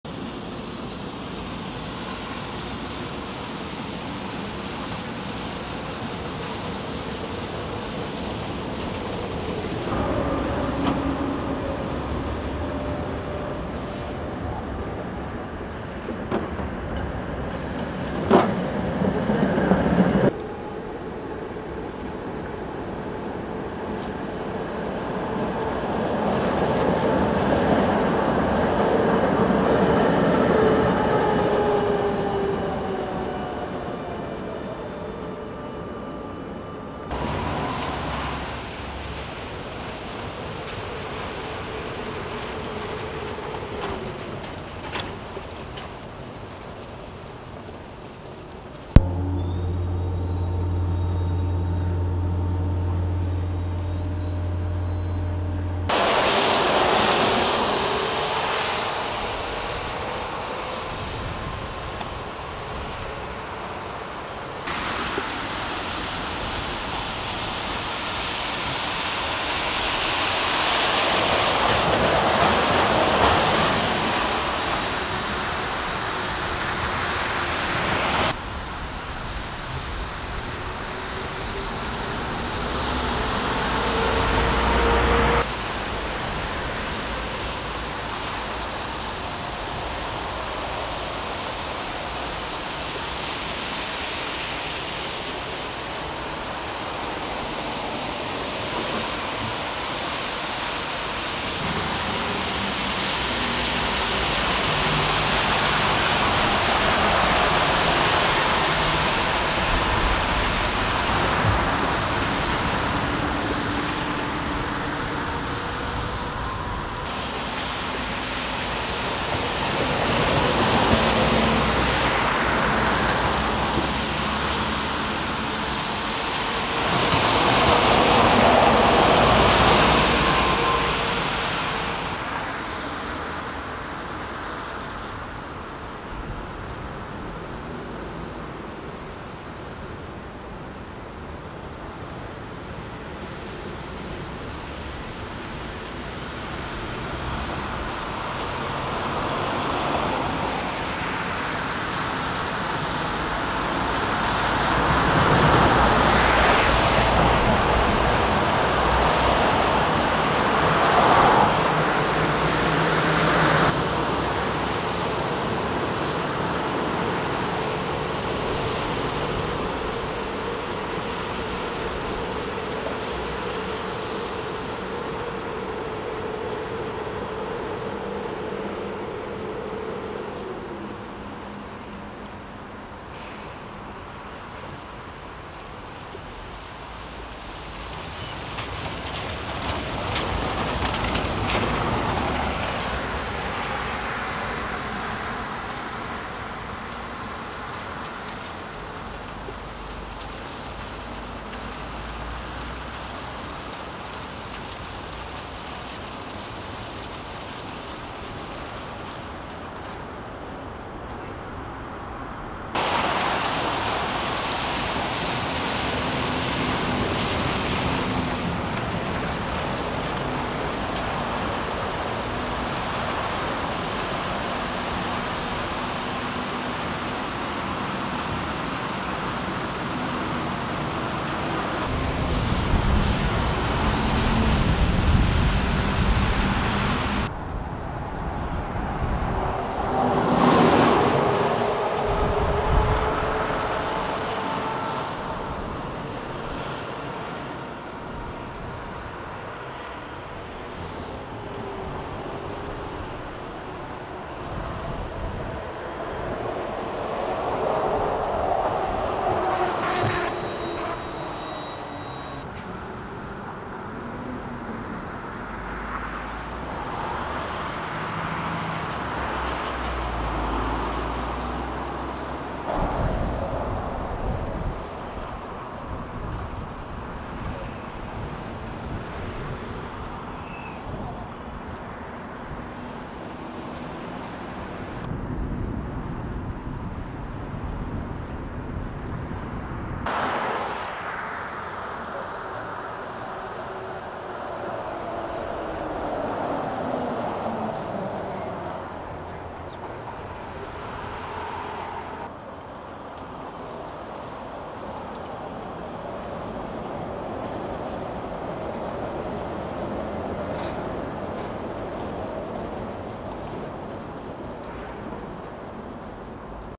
První den plného provozu na lince 58, 140 zrušena, 1. 2. 2024 | 8K HDR
Na lince se objevují trolejbusy SOR TNS18, Škoda 38Tr a naftové autobusy.